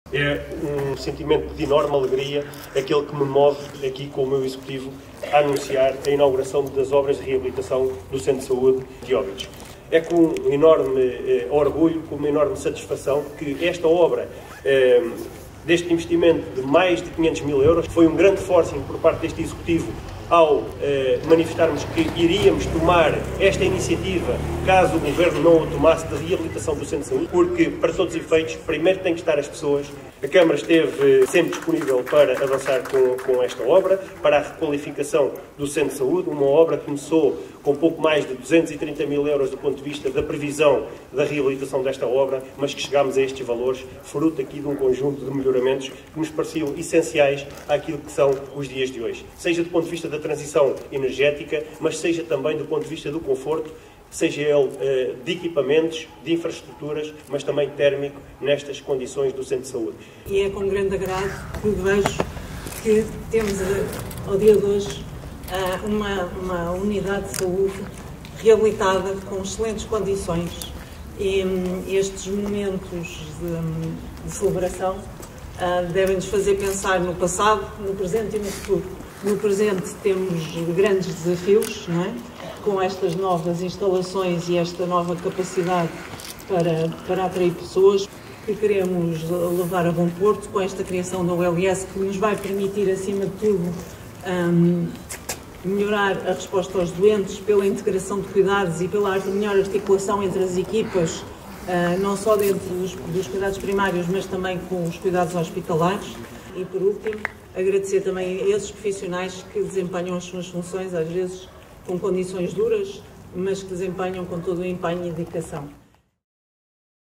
Inauguração da Unidade de Saúde • Feriado Municipal de Óbidos 2024
Dia 11 de Janeiro, quinta-feira, pelas 11 horas,foi inaugurado o renovado Centro de Saúde de Óbidos, que foi alvo de obras de requalificação. Trata-se de um investimento de 516 mil euros euros, que inclui obra de substituição de todo o piso.
O remodelado Centro de Saúde vai dispor de 15 gabinetes médicos, um espaço de atendimento ao público com três postos, uma sala de espera para utentes e uma sala para reuniões.